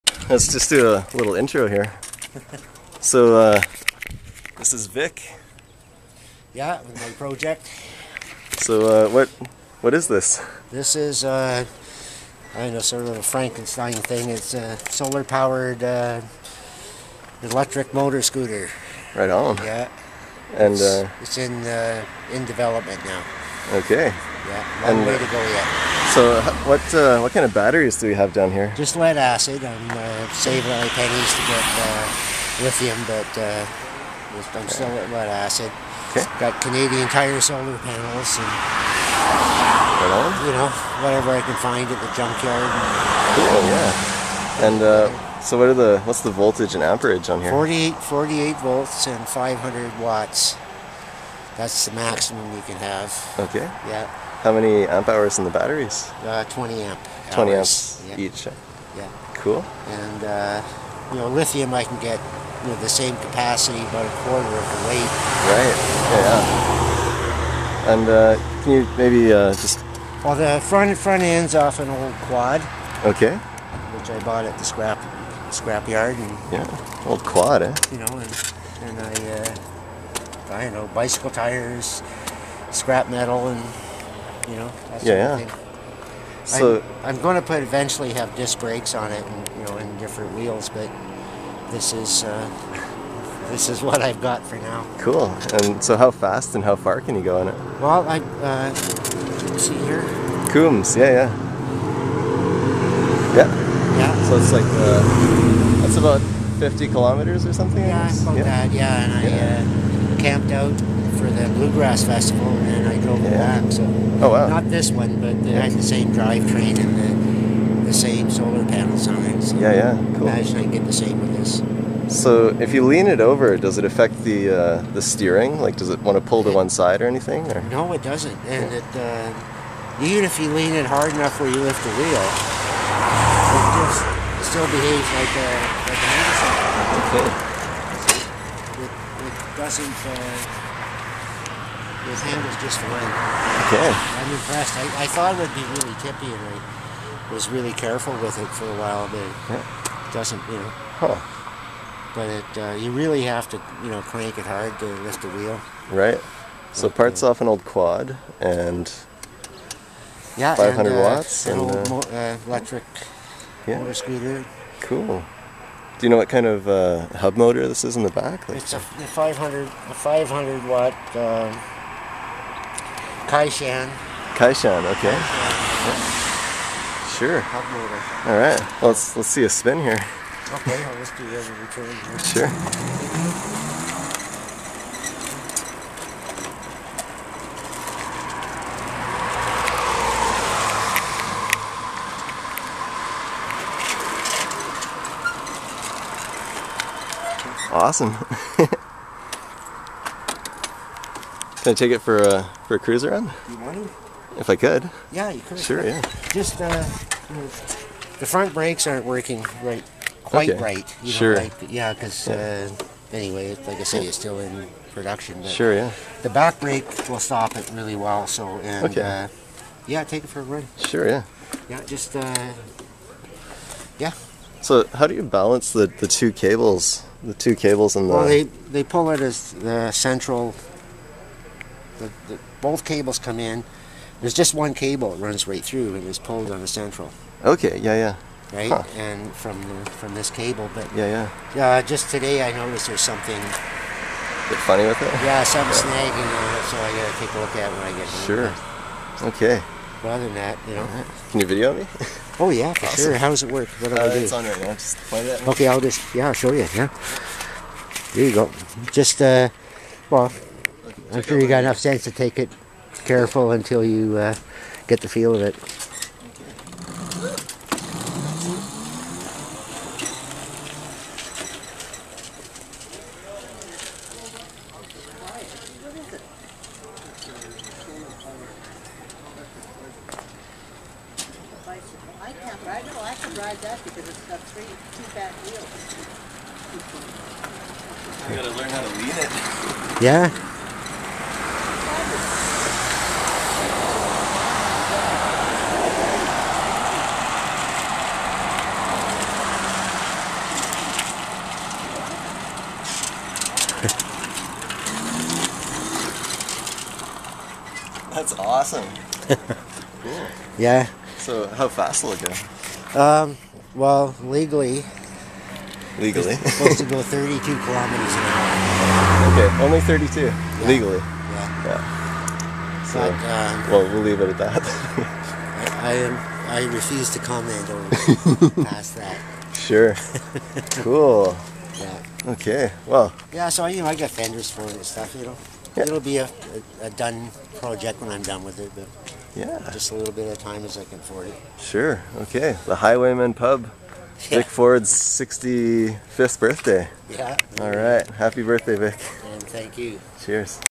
Solar Powered DIY Electric Trike 50+KM, 500watt motor, 48volts, 20amphours
Electric trike! This guy's hobby is to make electric vehicles pretty inexpensively. This one can charge by its solar panels so he leaves it sitting around in the yard to charge or he can plug it in. He wants to put on one more solar panel to get his full 48 volt charge. This things goes pretty fast and far for something so cheap to build.

So don't let anyone tell you that electric vehicles are impractical or too expensive. Just pick up a wrench and make it happen.